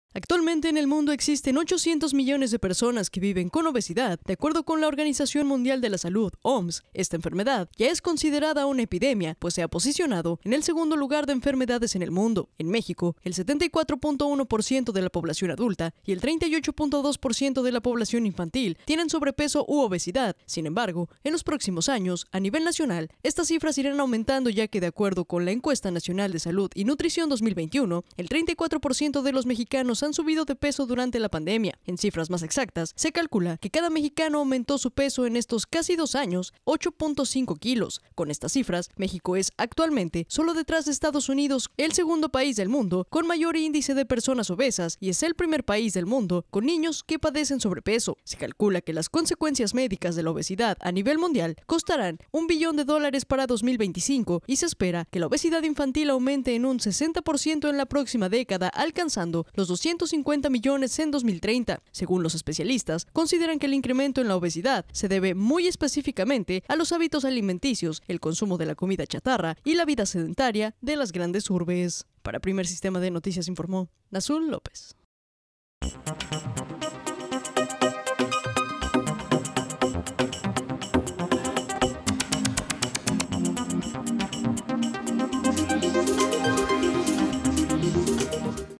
Hoy se conmemora el día mundial contra la obesidad.
Hoy se conmemora el día mundial contra la obesidad, en México el 74.1 por ciento de la población adulta tiene esta enfermedad.